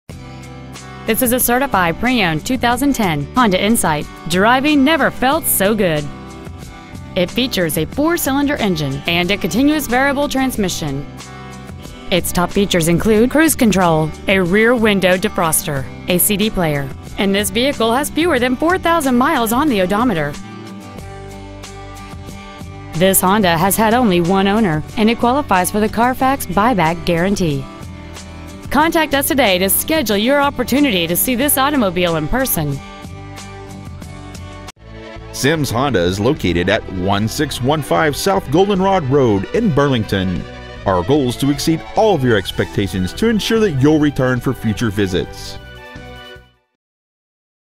Certified 2010 Honda Insight Bellingham WA 98226
We are proud to present this 2010 Honda Insight .

 Year : 2010
 Make : Honda
 Model : Insight
 Engine : 4 cyl
 Trans . : CVT
 Exterior : Gray
 Miles : 3,946
 Interior : Gray
 Sims Honda
 1615 S. Goldenrod Road
 Bellingham , WA 98233
 2010 Honda Insight Bellingham WA